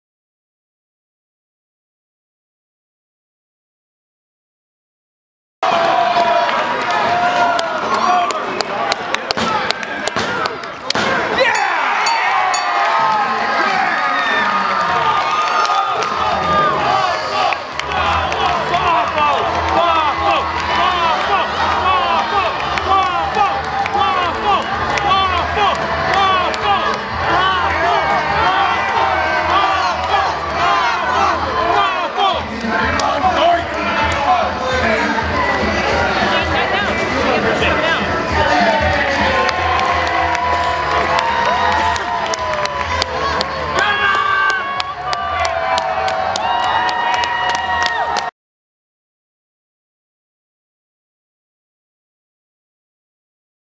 Pro Wrestling: "Waffles" Vs. Doink The Clown
Young rookie wrestler "Waffles" aka Anthony Farley scores a fluke victory over pro wrestling legend Doink the Clown by pinning the clown's shoulders for a three count while attempting to force feed him a waffle. The awestruck audience wildly cheers for the erstwhile jobber after his stunning upset victory and Doink takes the humiliating loss in stride. From Maximum Force Wrestling's "South County Invasion" event held on April 25, 2015 at Jamestown Community College in Jamestown, New York.